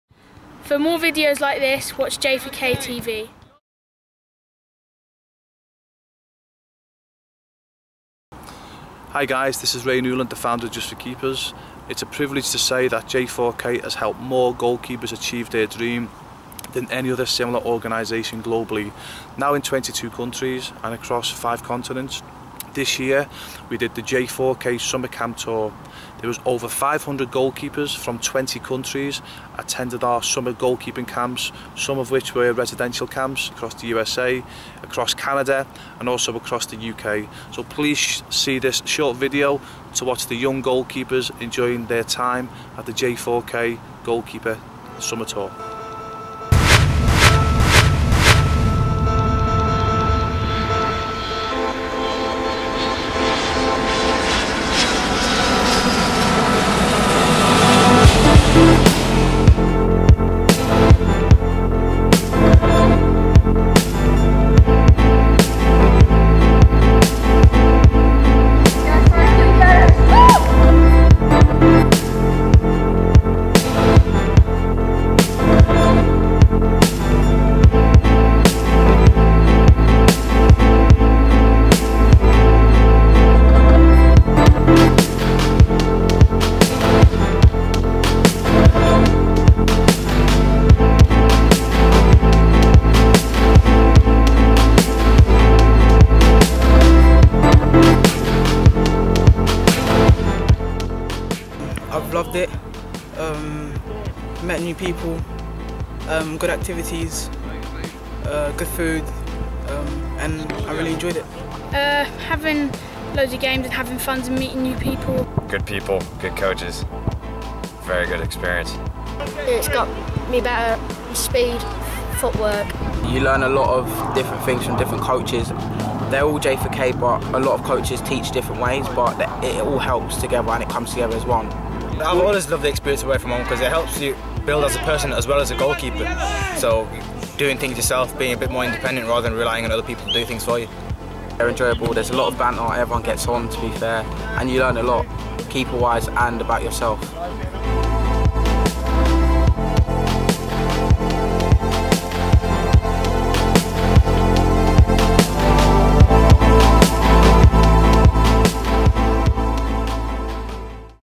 Goalkeeper Training - J4K Summer Tour 2014
FREE GOALKEEPER SESSION

We look back at J4K's summer tour with this short promo. From USA to CANADA then on to the UK. A fantastic tour with 500 goalkeepers attending from 20 countries...